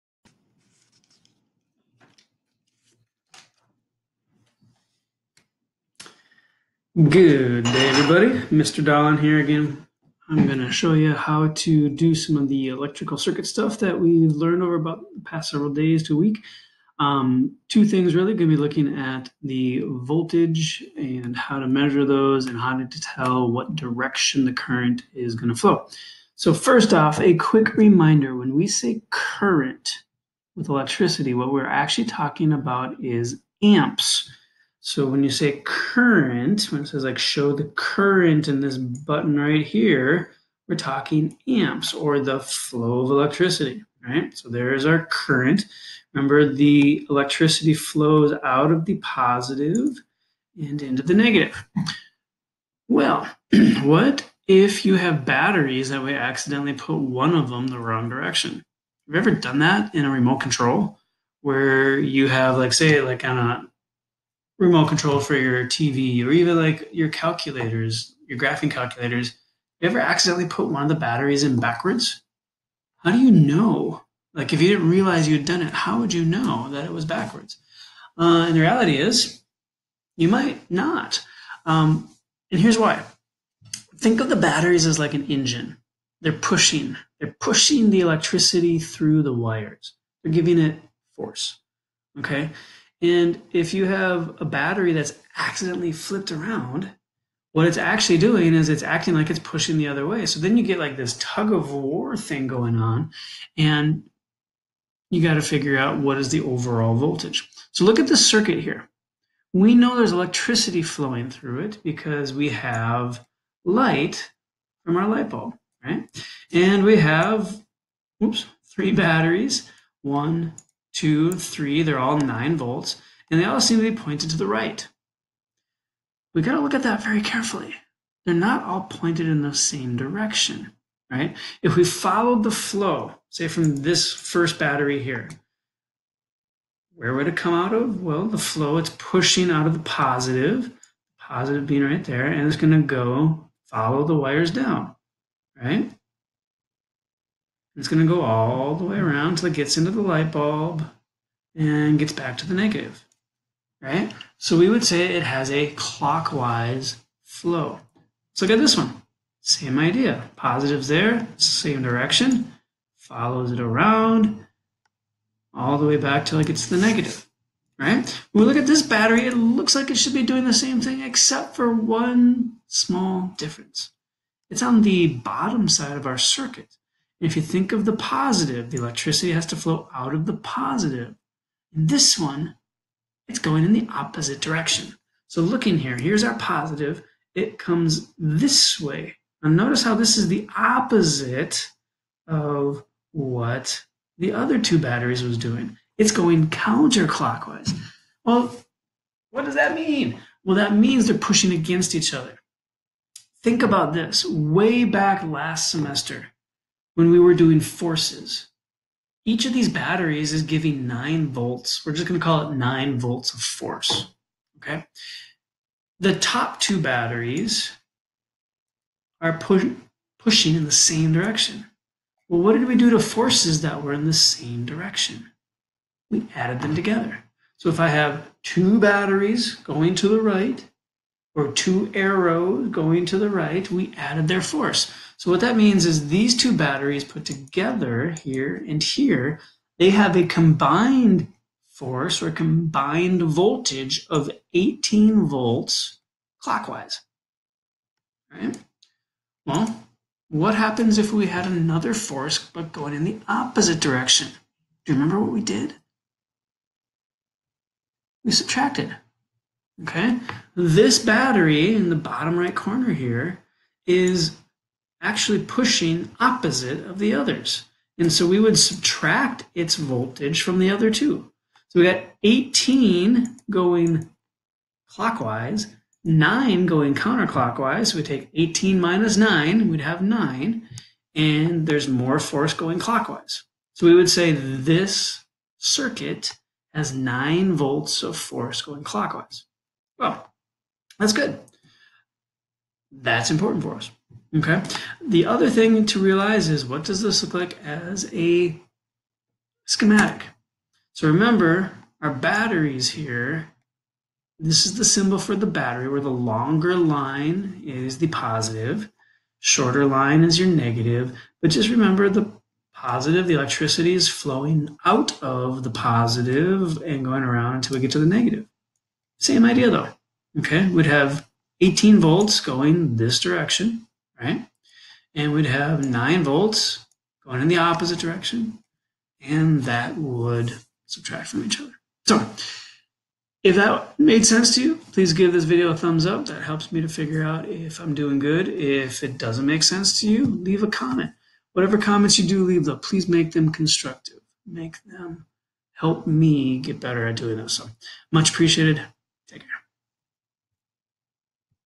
Finding the total voltage of a circuit
Circuit Voltage:

Ever wonder why batteries in a remote control or your graphing calculators have to go in a specific direction?  This video will explain that.

When you have a circuit powered by multiple batteries, how do we determine the amount of voltage available to that circuit? This video will show you what to do.